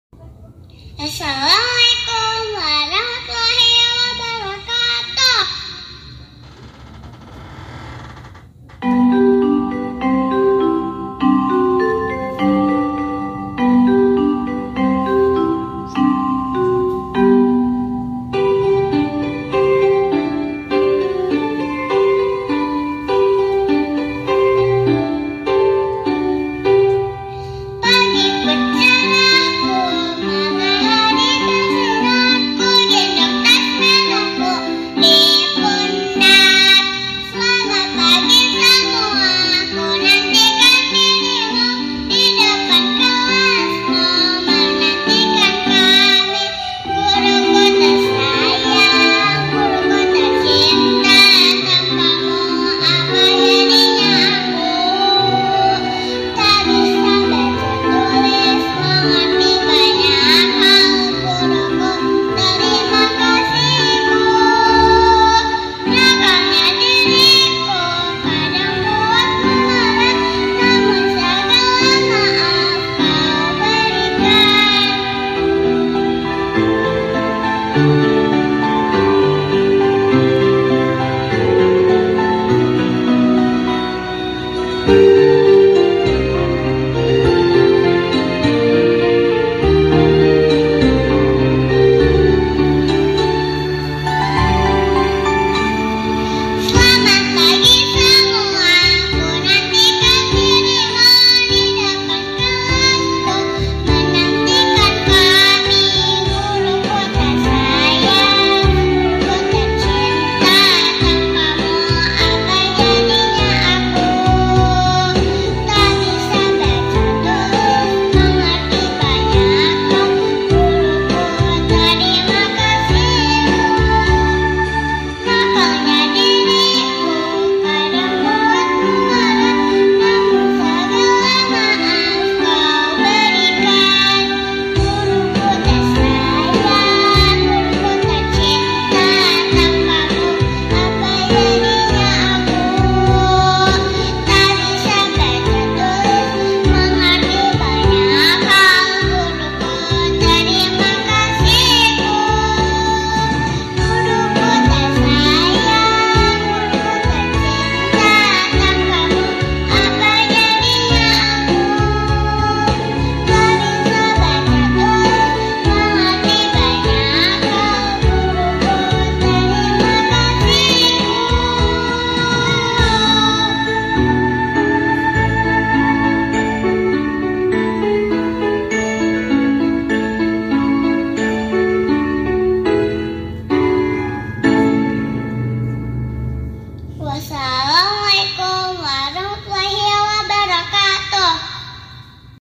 AKSERA VIRTUAL 2021_LOMBA KARAOKE_AYSHI FARHA_RA PERSIS 169 ARRAHMAN KEC.PAMEUNGPEUK KAB BANDUNG
Mari kita dukung dalam mengembangkan potensi yang dimiliki anak didik kita salah satunya dengan mengikuti AKSERA VIRTUAL PW IGRA JABAR 2021